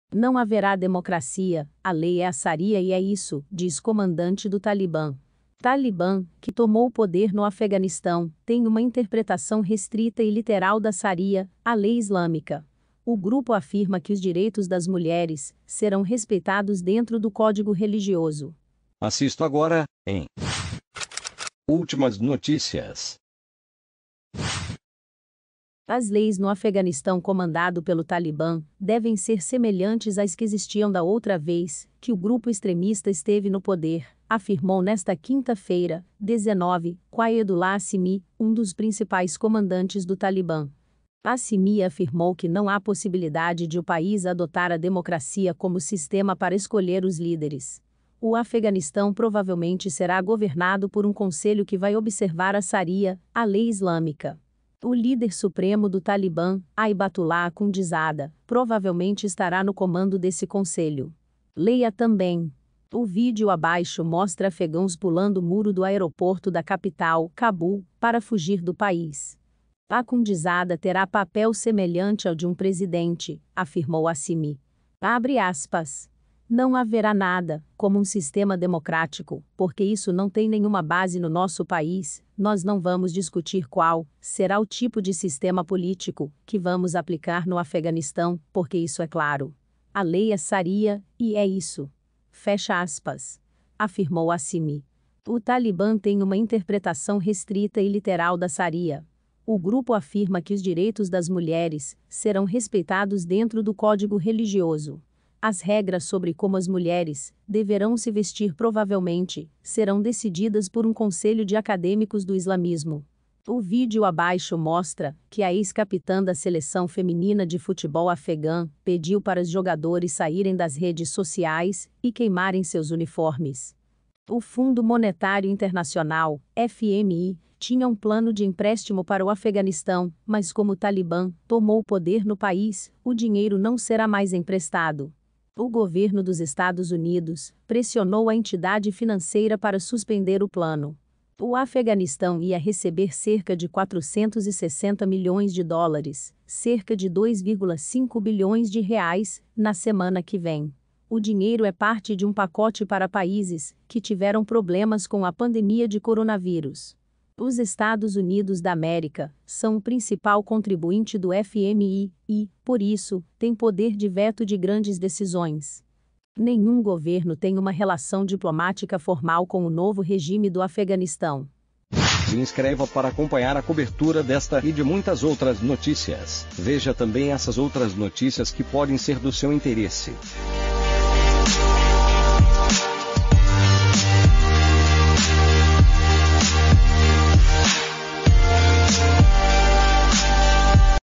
'Não haverá democracia, a lei é a sharia e é isso', diz comandante do Talibã
Talibã, que tomou o poder no Afeganistão, tem uma interpretação restrita e literal da sharia, a lei islâmica. O grupo afirma que os direitos das mulheres serão respeitados dentro do código religioso.

🔴 Quer GANHAR MAIS DE R$10.000,00 todo mês na internet?

Acessado em: 19 de agosto de 2021 às 13:20